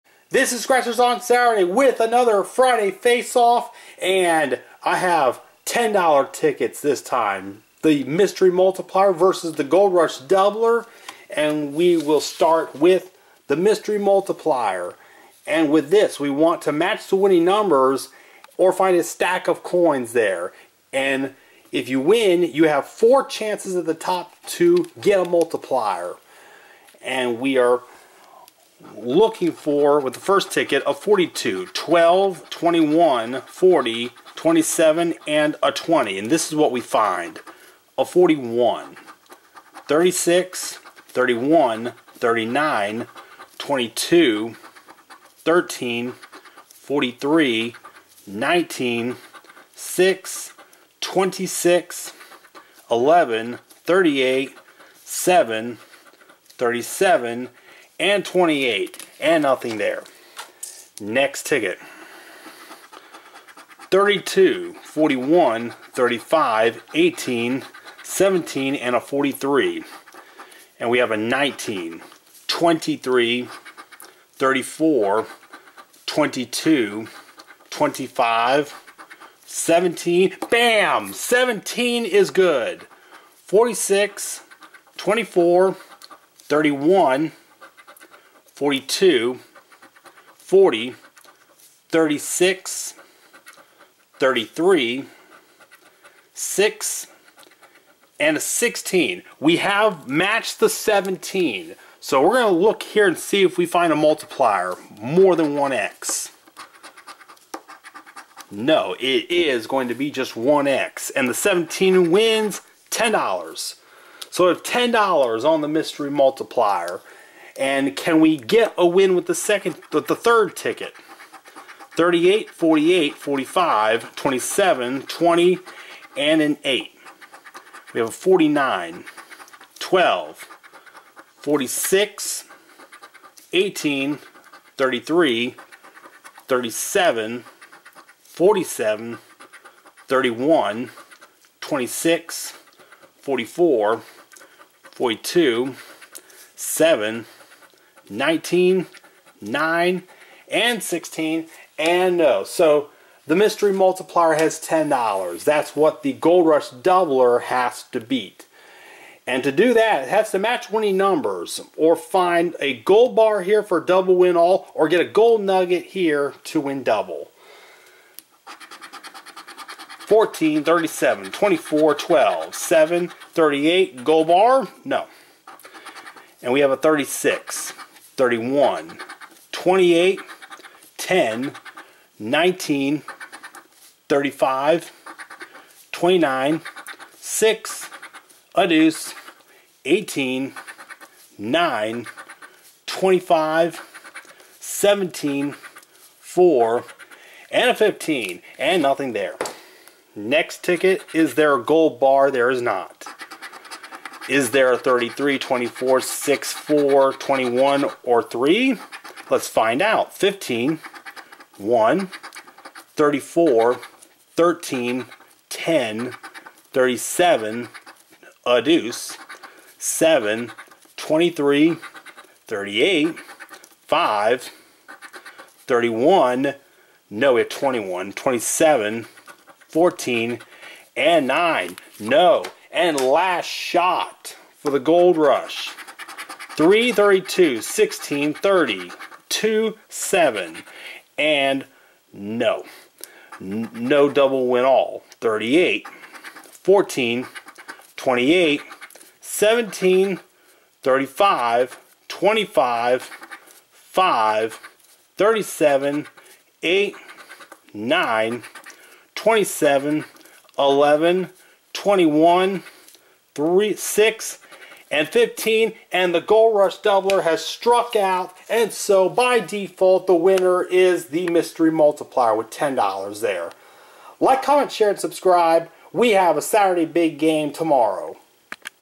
FRI. FACE-OFF 24: $10 MYSTERY MULTIPLIER VS. GOLD RUSH DOUBLER Florida Lottery Scratchers
Florida Lottery scratch off tickets featured:
   $10 Mystery Multiplier (3)
   $10 Gold Rush Doubler (3)